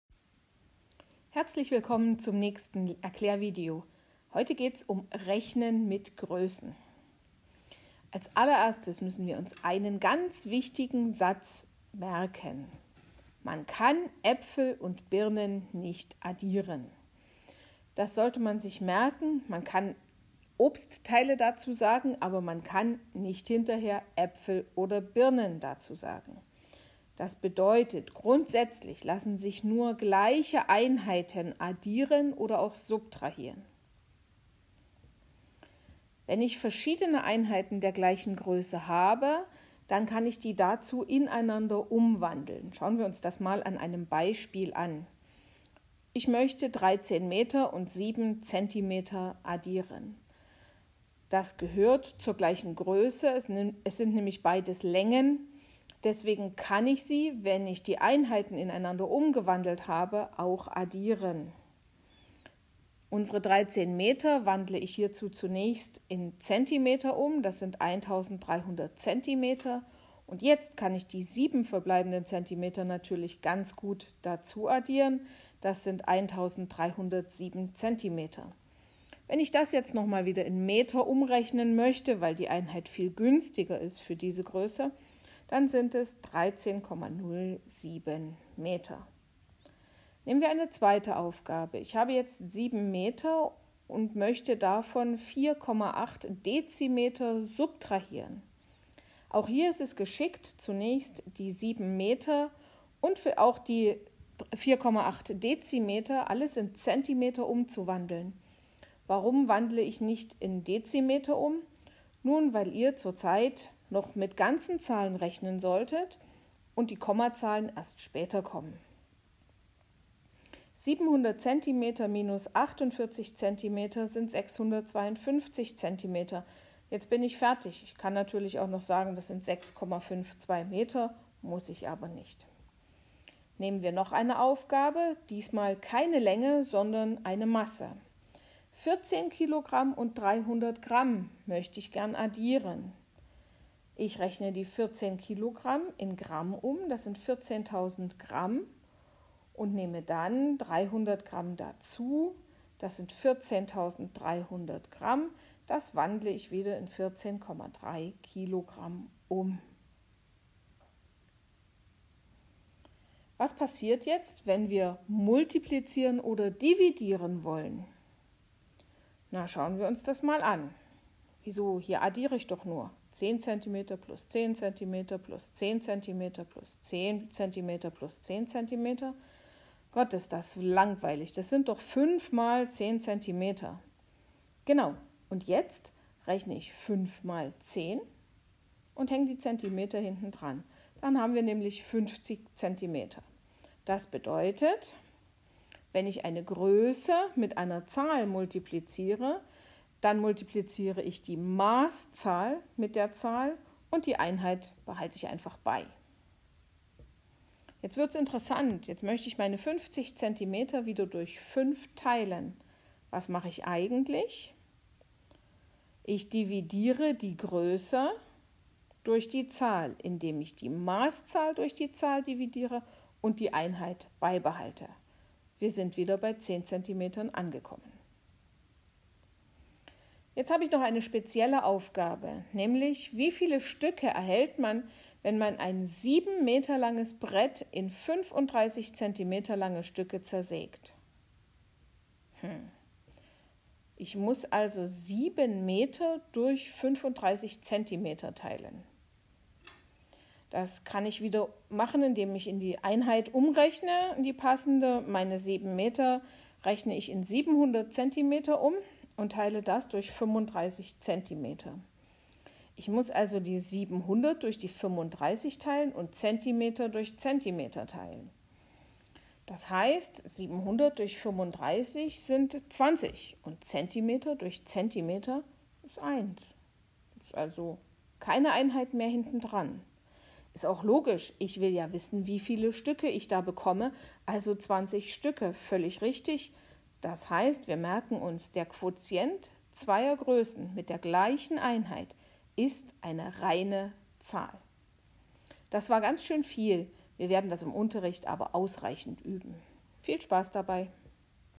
M 5 5 3 Rechnen mit Größen1
In diesem Video lernst du, wie man mit Größen rechnet und umrechnet.